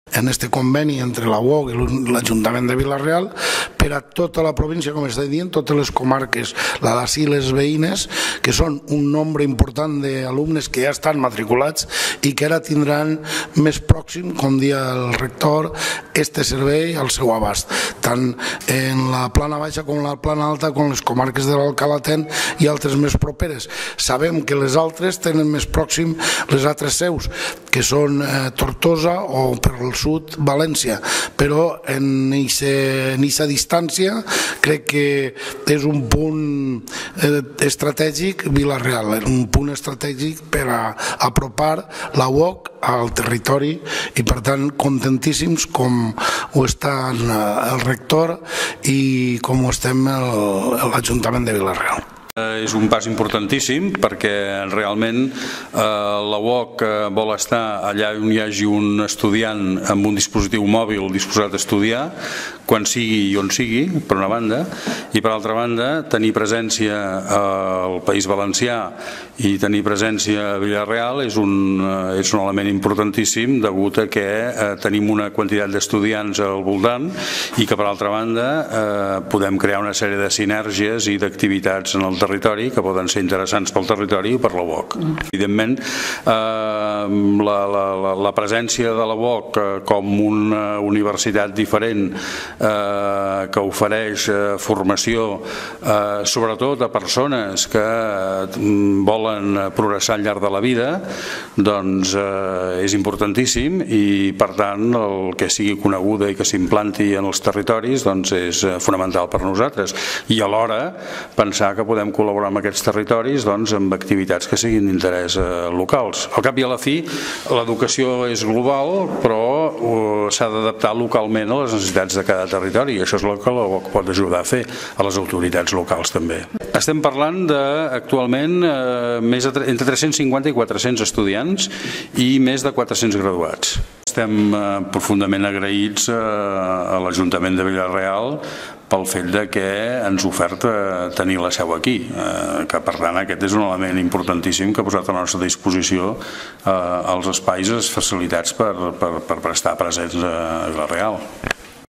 La sede la UOC en Villarreal amplía la oferta universitaria
El alcalde de Villarreal, José Benlloch, y el rector de la Universitat Oberta de Catalunya (UOC), Josep A. Planell, han firmado el convenio por el cual el Ayuntamiento cede gratuitamente el uso de un espacio en la Biblioteca Universitària del Coneixement, ubicada en la avenida Pius XII, para la puesta en marcha de la sede de la UOC en Villarreal con el objetivo de promover entre la ciudadanía la formación universitaria y ofrecer servicios de asesoramiento y gestión académica al colectivo de estudiantes de la ciudad y comarca.